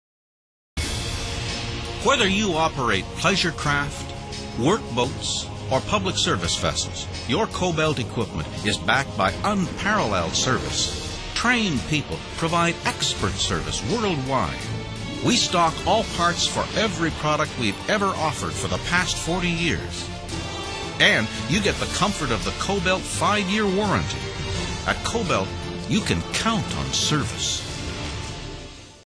Kobelt Service from Marine Specialties
Whether you operate a pleasure craft or work boat, you can depend on Kobelt marine products. They stock parts for all their products made since 1962. You can count on Kobelt service.
Please note: in this video it states that Kobelt's warranty is 5 years which has now been changed to 2 years